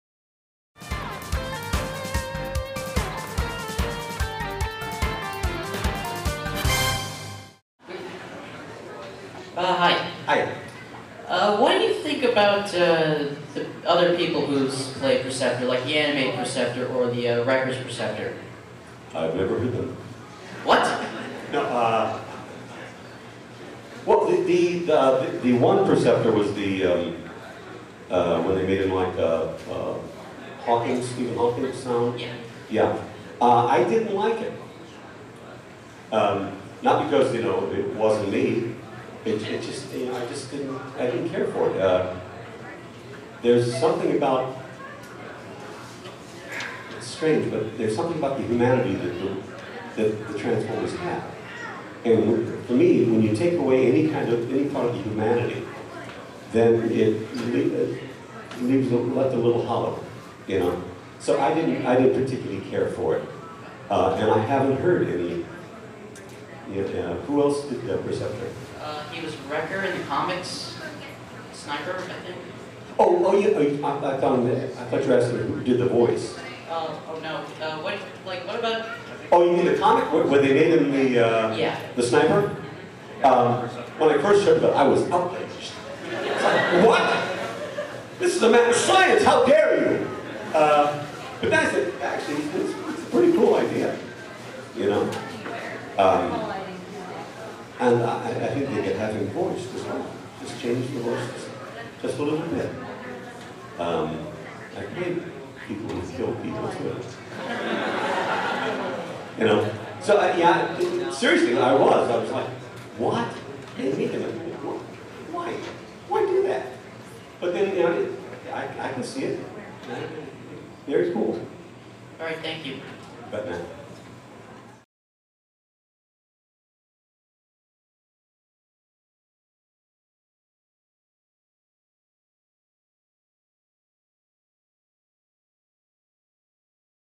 What Transformers G1 Perceptor Actor Paul Eiding Thinks of Modern Perceptor.
During TFcon Toronto, voice actor Paul Eiding (Ben 10, Transformers Generation One) shares his feelings on more recent renditions of Perceptor, a character that he originally helped bring to life.